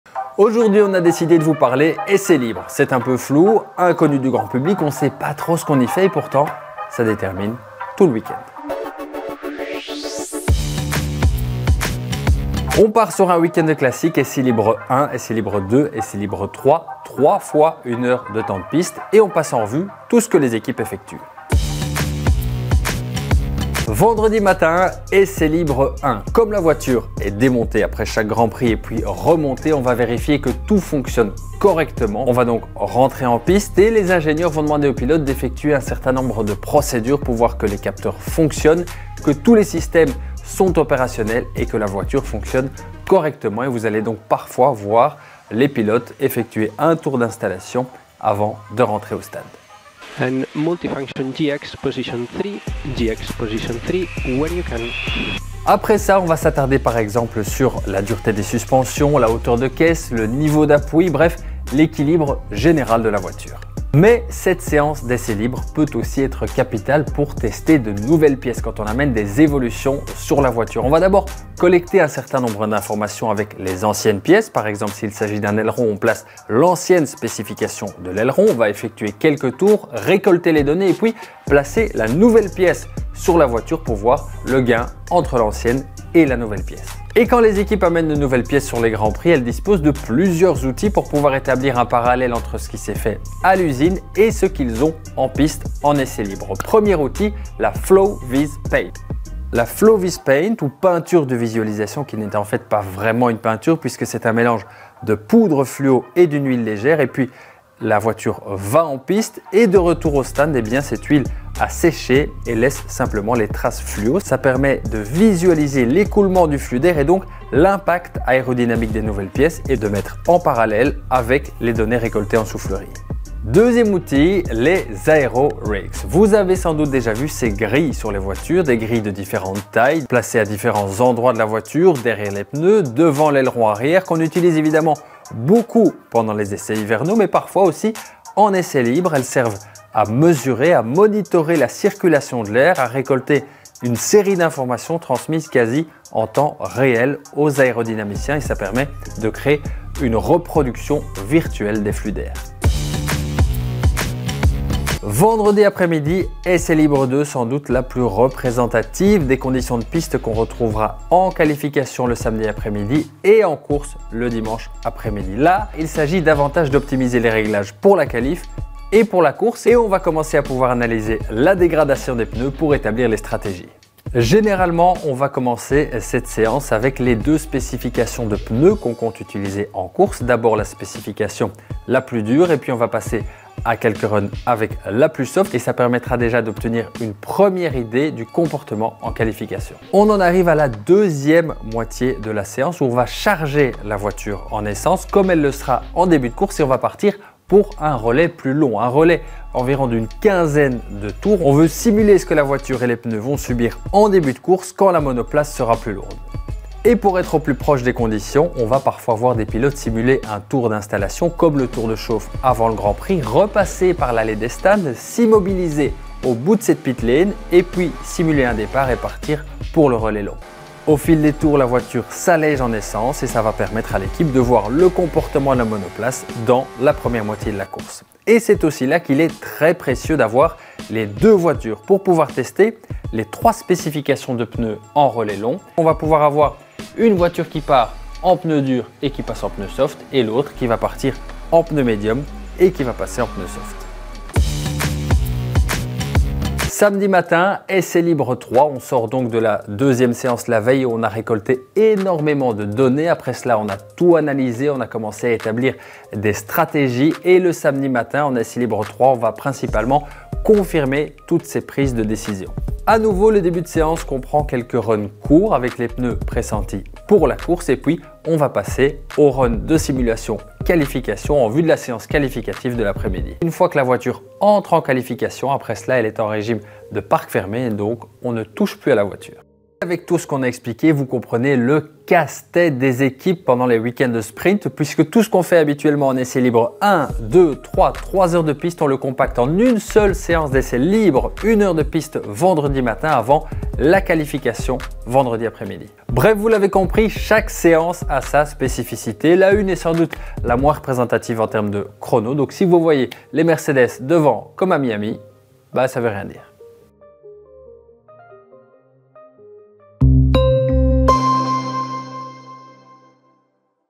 À quoi servent les essais libres en F1 ? | F1 Basics
🤔 Mais que font donc les équipes durant les essais libres d’une manche du championnat du monde de Formule 1 ?
👉 Simple entraînement avant les qualifications et la course ? Moment un peu flou, compliqué à décrypter et qui ne revêt pas une grande utilité ? Détrompez-vous ! Ce temps de piste est capital pour les équipes, car il oriente tout le week-end l’évolution de la monoplace au cours de la saison.
Chaque séance a un but bien différent et les week-end de courses sprint, où les teams ne disposent que d’une seule session d’une heure le vendredi matin, représentent un véritable casse-tête.

🤓 On vous explique cela dans ce numéro de F1 Basics.